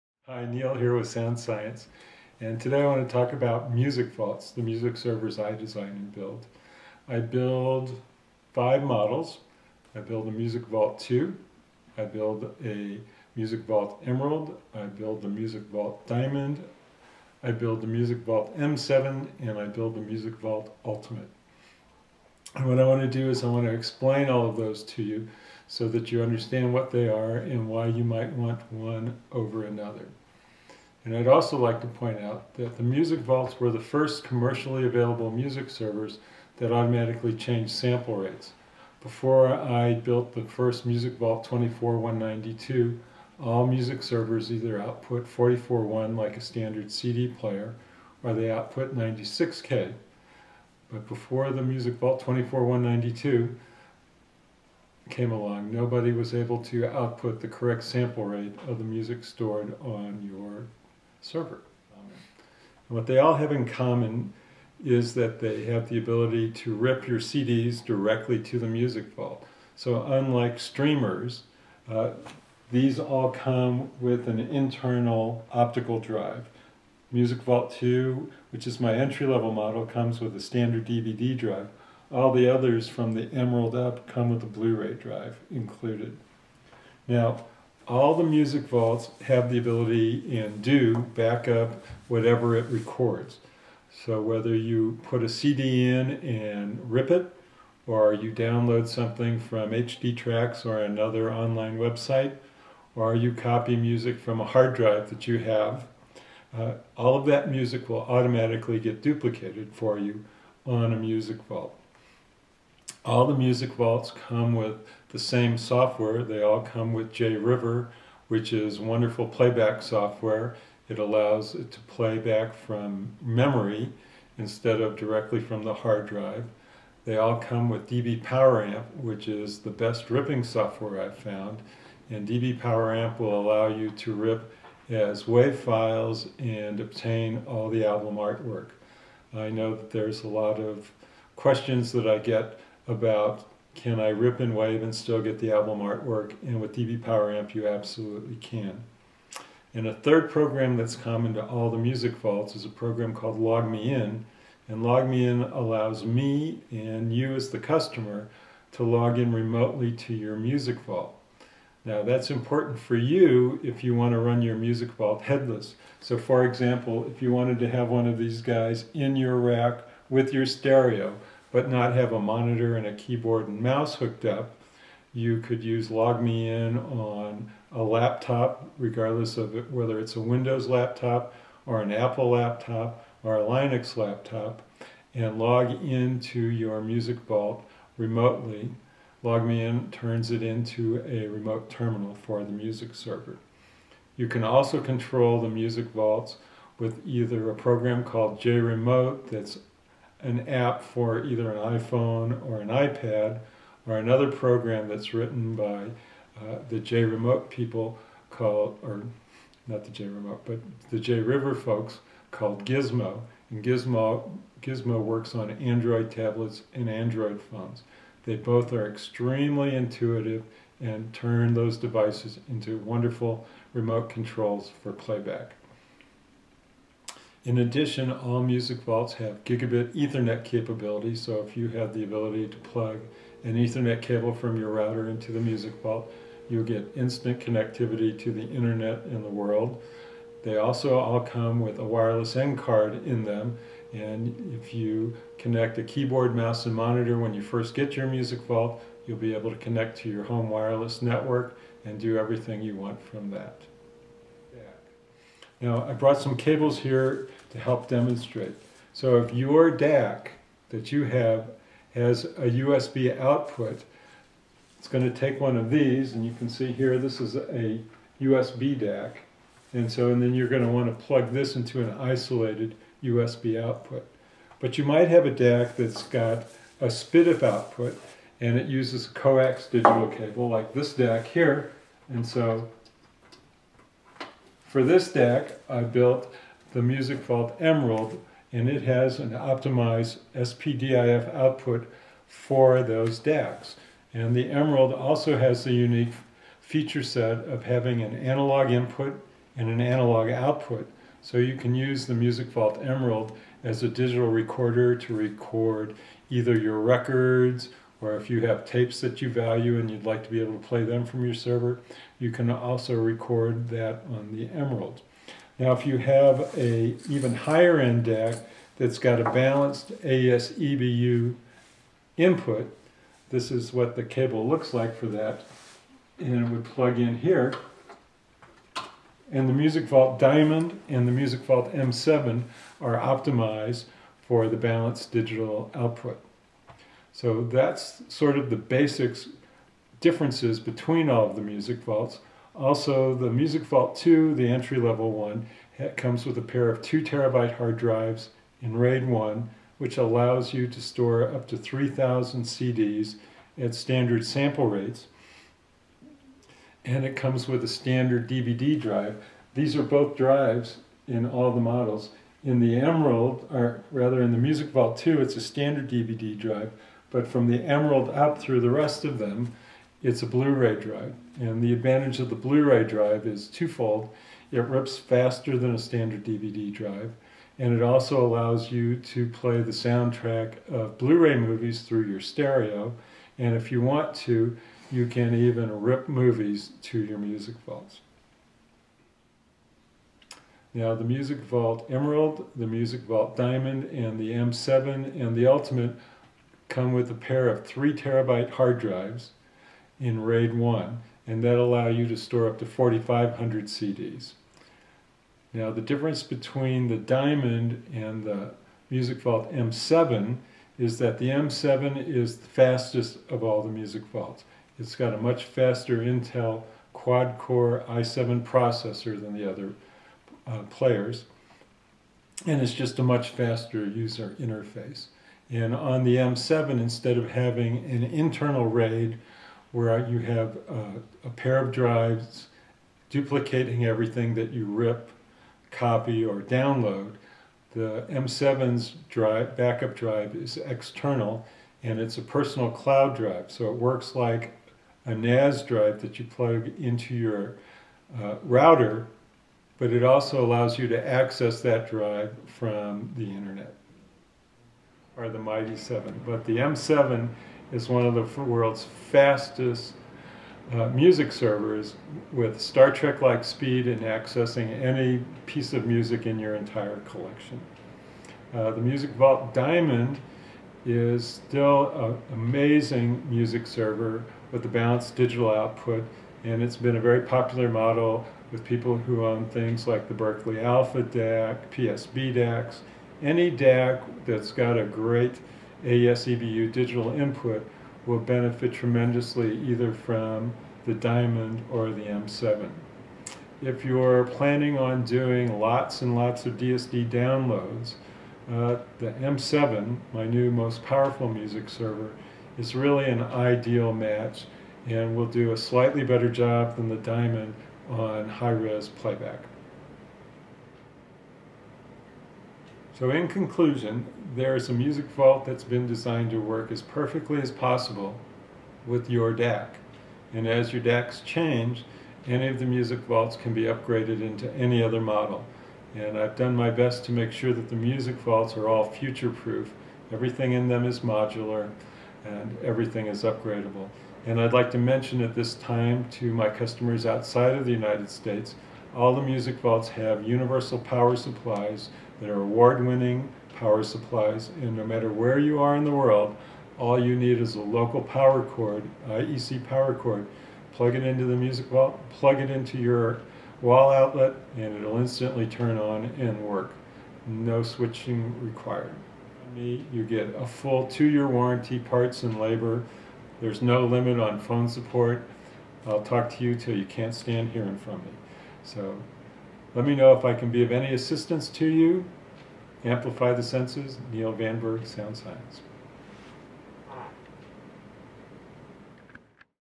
Sound Science Music Vault Overview, 2013
This video describes the Sound Science Music Vault.  Neal Van Berg talks about the similarities and differences between the Music Vault II, the Emerald, the Diamond, the M7, and the Ultimate.  He also talks about which Music Vault is appropriate for which DAC.

The Music Vault II has won the Absolute Sound Editor's Choice Award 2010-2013.